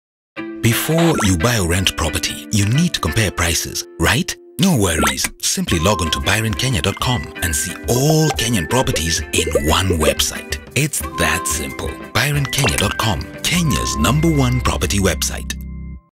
BuyRentKenya 15secs Brand TV Commercial
Before you buy or rent property, you need to compare the prices. BuyRentKenya offers you an opportunity to properties from across Kenya by simply visiting